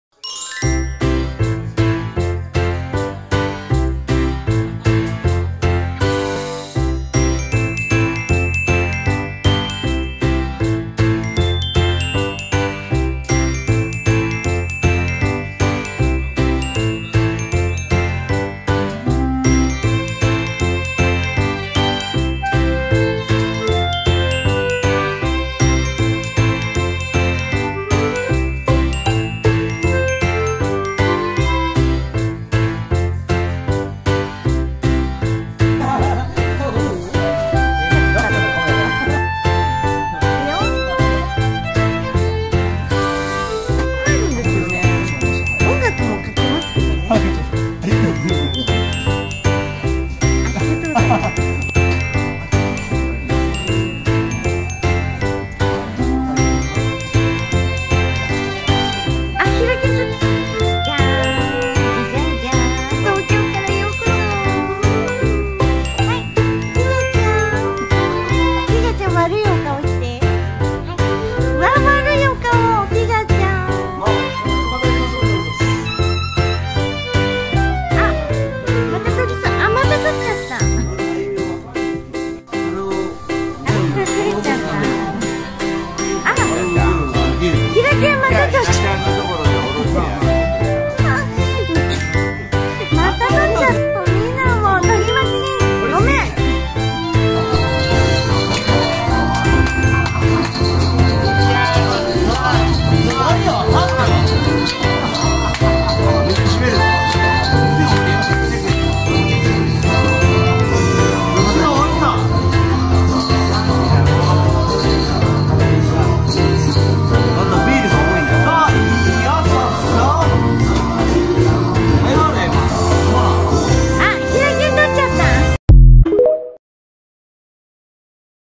@キッズハウス映像日記2024年3月火曜日♥スタッフは花音ちゃん
火曜日キッズ、ダーツに燃えるお仲間たち、カラオケを熱唱するお仲間たち、トークに花を咲かせるお仲間たち、はしゃぐお仲間たち、
みんな違って、みんないい♥